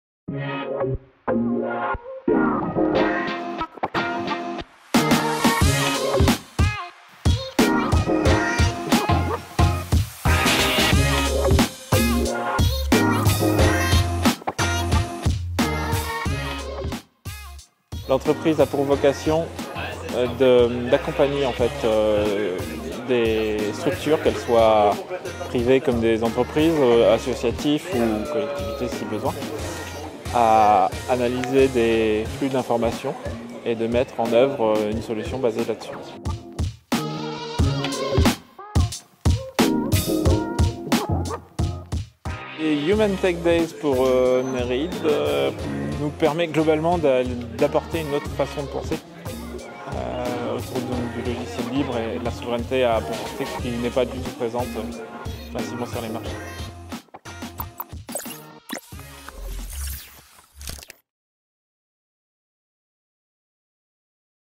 Nicolas MALIN - Human Tech Days Live Tour 2022
Interview de Nicolas MALIN, Consultant chez Néréide à l'occasion de sa participation au Human Tech Days Live Tour 2022 - étape de Bourges le 8 juin 2022.